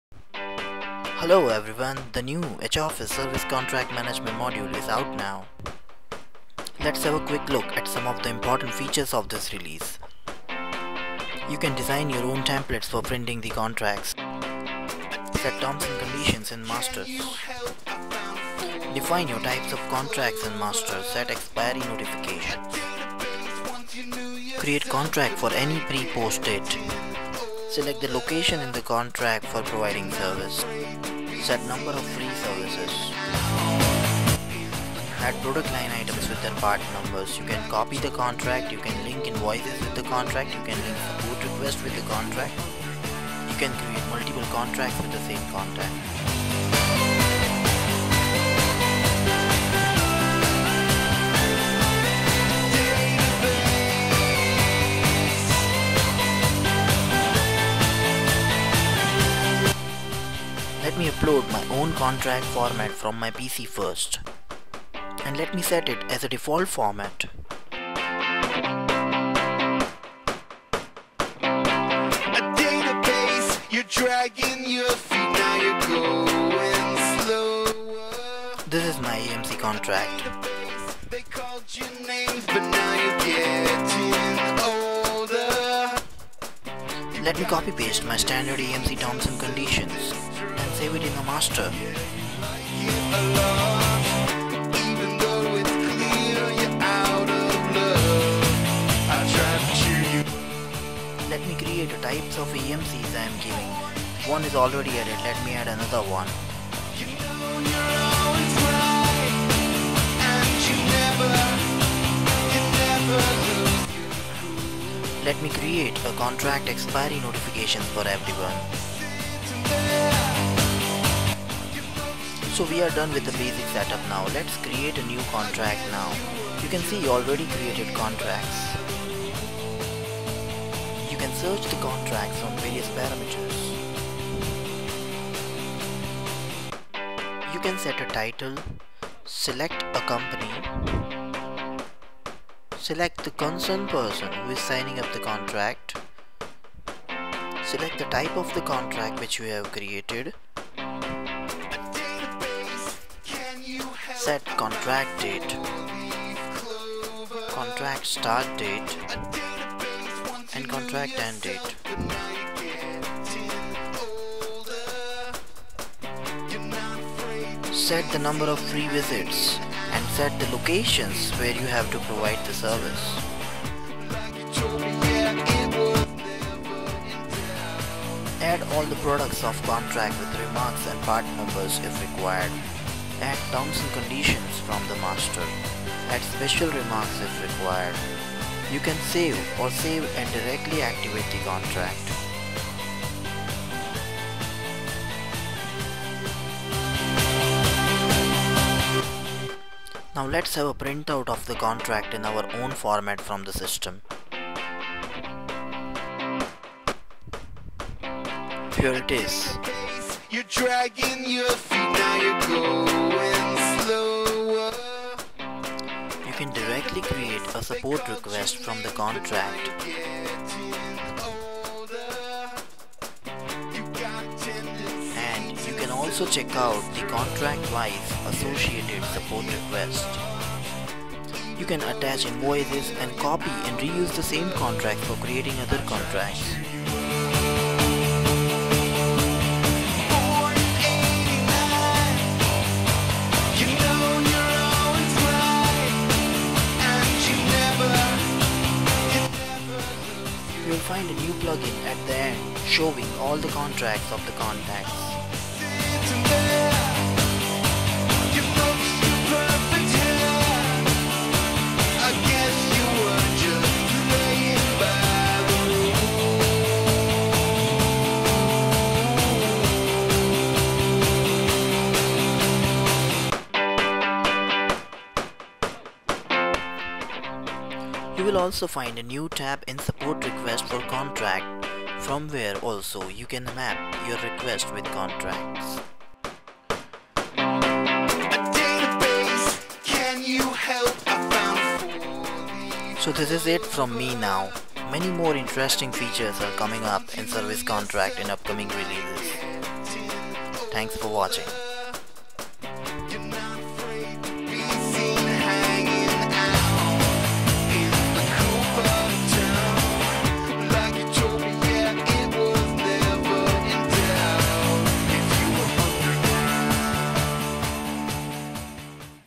Amazing H-Office Service Contract Management Module Introcution
This video is presenting the new service / AMC contract management module features of amazing h-office software.